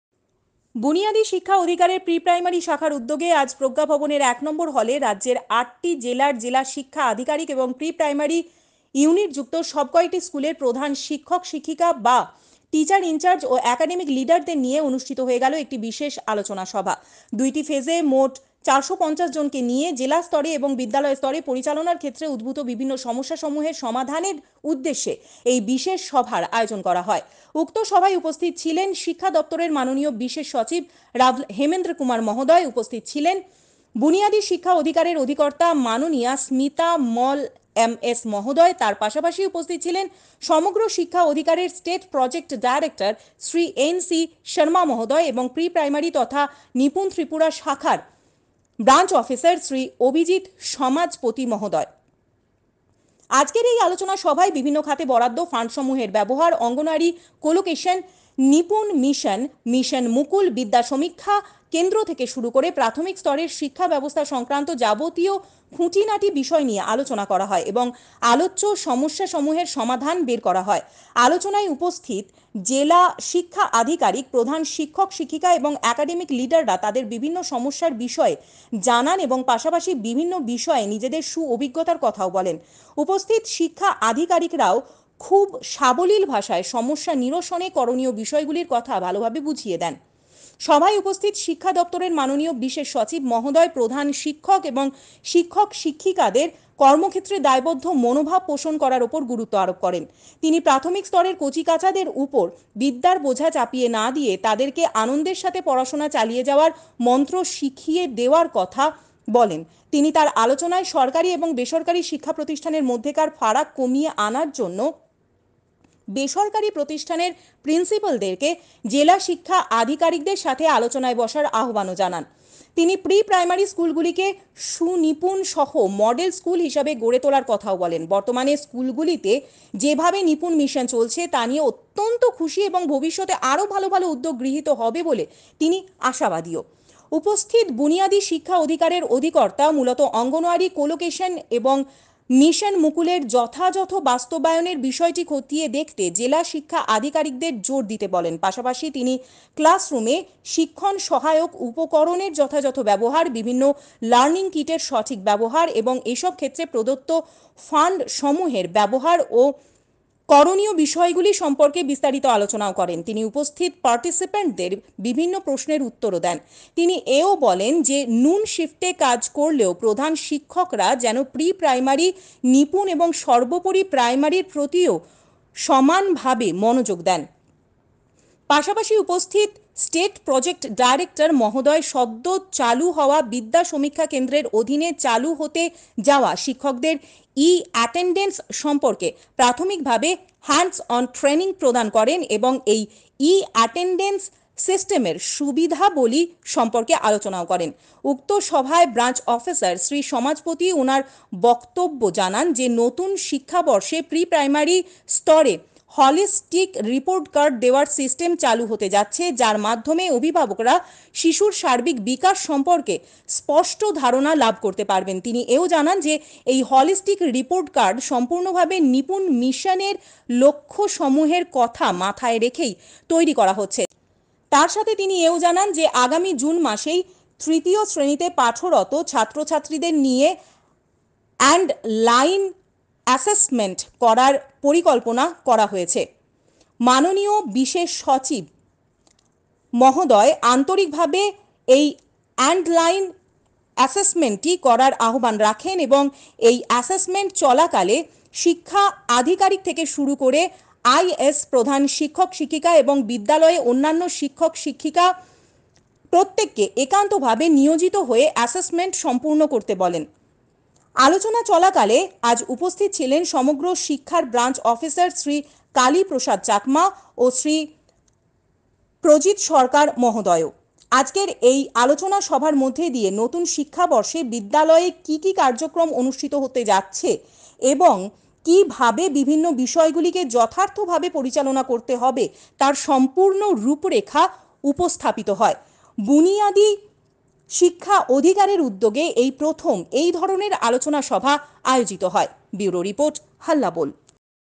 প্রজ্ঞাভবনে জেলা স্তরে ও বিদ্যালয় স্তরে প্রাথমিক শিক্ষা সংক্রান্ত বিভিন্ন সমস্যার সমাধান নিয়ে আলোচনা
প্রজ্ঞাভবনে জেলা স্তরে ও বিদ্যালয় স্তরে প্রাথমিক শিক্ষা সংক্রান্ত বিভিন্ন সমস্যার সমাধান মূলক আলোচনা হয় প্রধান শিক্ষক ও শিক্ষকা সহ শিক্ষা সচিবের উপস্থিতিতে।